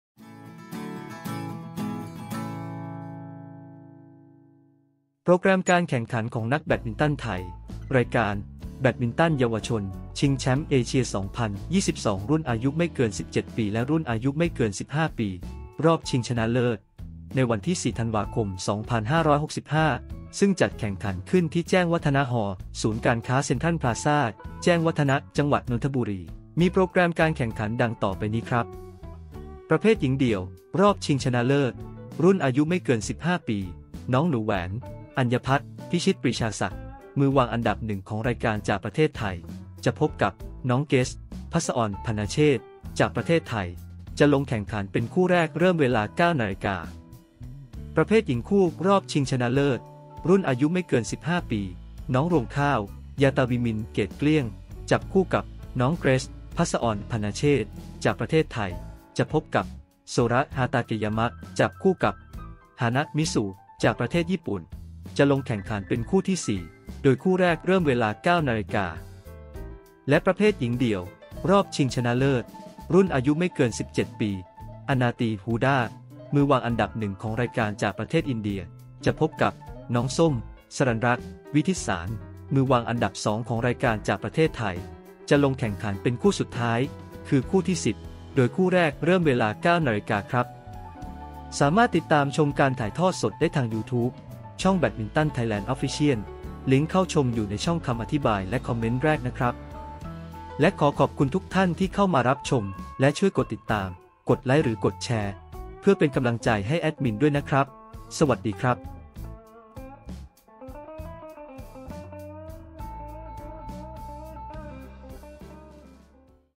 04/12/2022 | โปรแกรมนักแบดฯไทย | รอบชิงชนะเลิศ | เยาวชนชิงแชมป์เอเชีย 2022 รุ่น U17, U15 | +ลิ้งชมสด
04/12/2022 | โปรแกรมนักแบดฯไทย | รอบชิงชนะเลิศ | เยาวชนชิงแชมป์เอเชีย 2022 รุ่น U17 และ รุ่น U15 | +ลิ้งชมสด | @นักกีฬาไทย_HERO_IDOLS